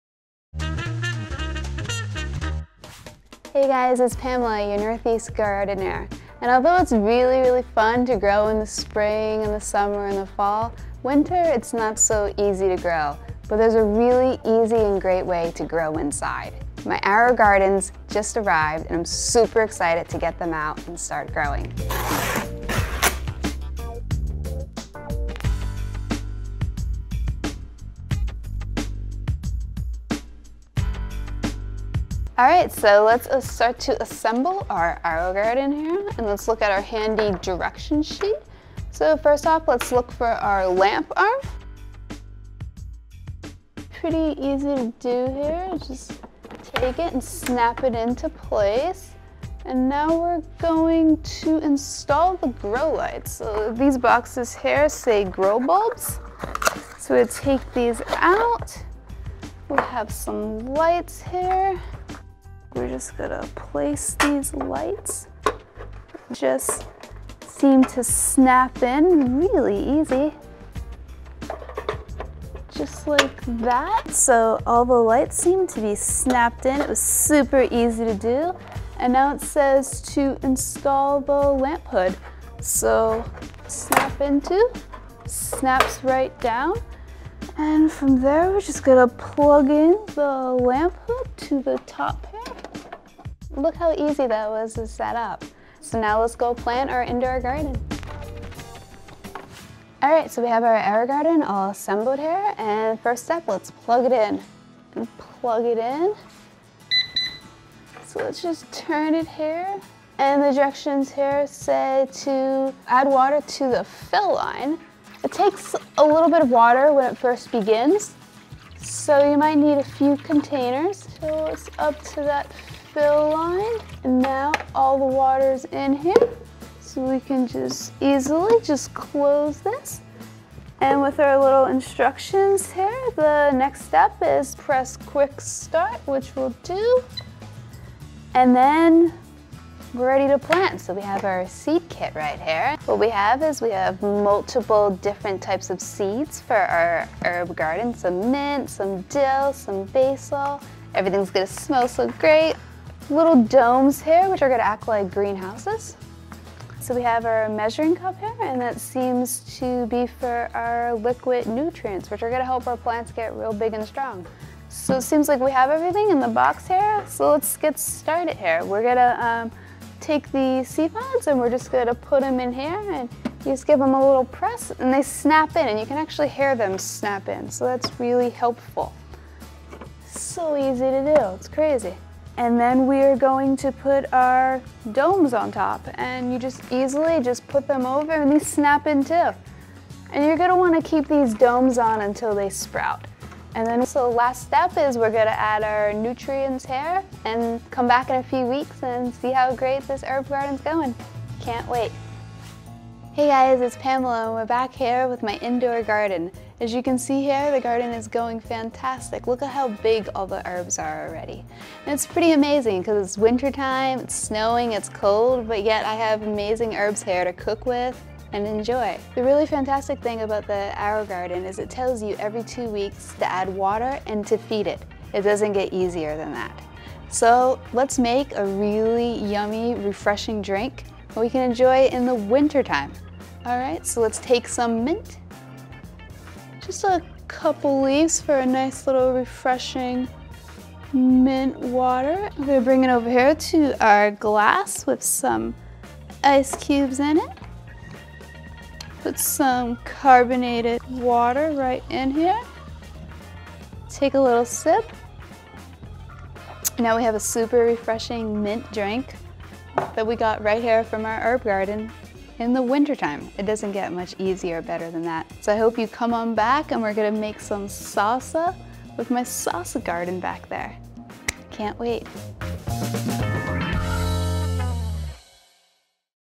AeroGarden - Easy to Set up and Plant with Pamela Reed
See how easy it is to set up and plant your AeroGarden! Pamela Reed takes you through step-by-step instructions.